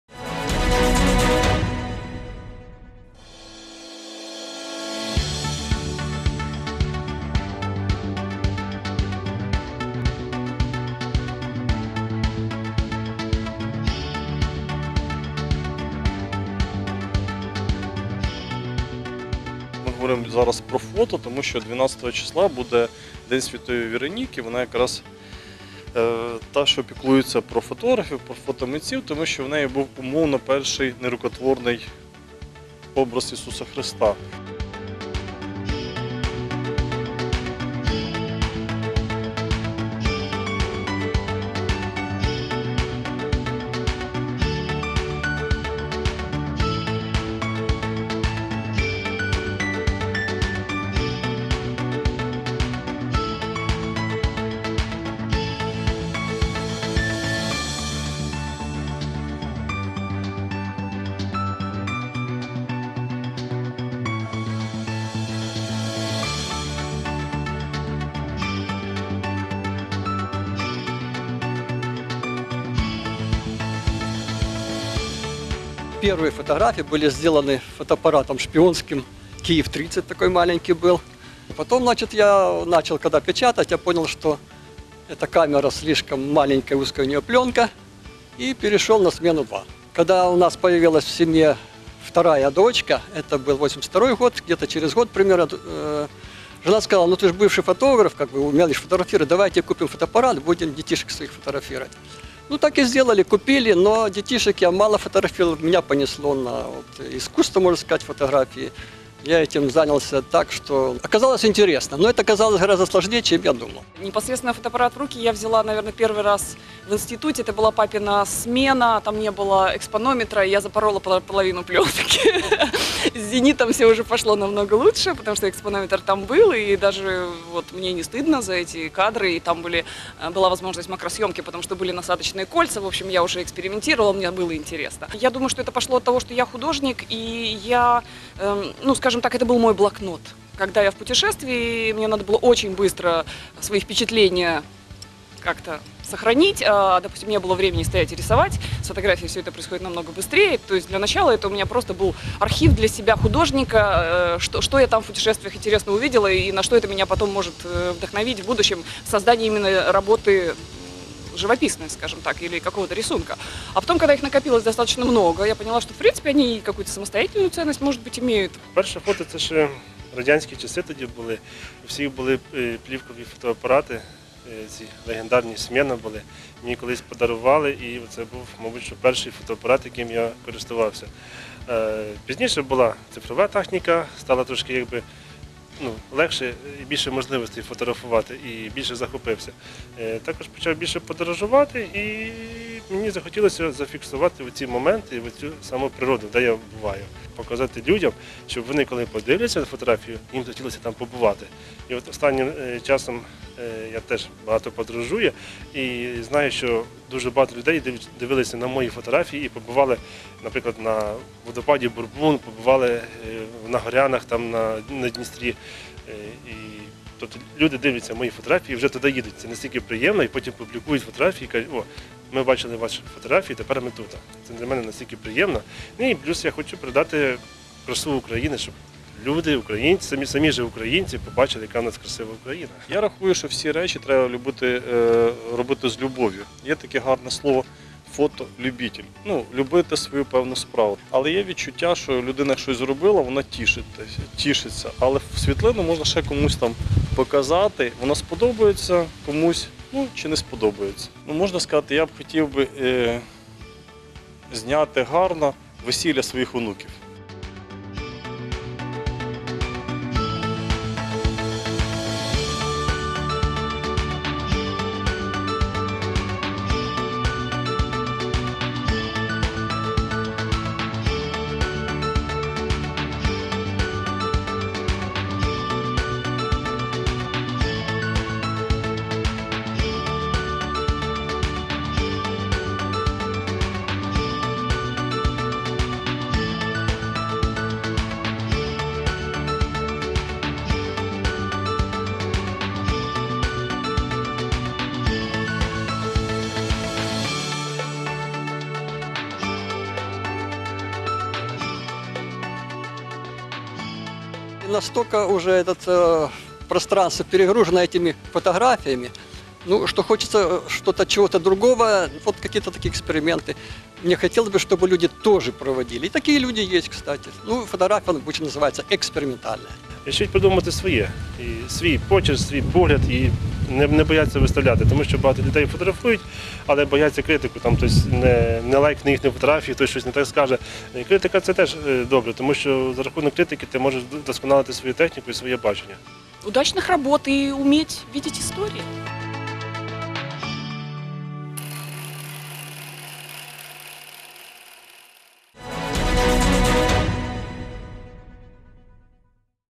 12 липня - День фотографа
12 липня в Україні відзначають День фотографа.
Існують дві версії виникнення цього свята.Згідно першої - в цей день вшановують Святу Вероніку- заступницю фотографів.
За іншою версією 12 липня присвячується всім фотографам на честь дня народження Джорджа Істмана. Саме засновник компанії Kodak зробив фотознімкування доступним для всіх охочих.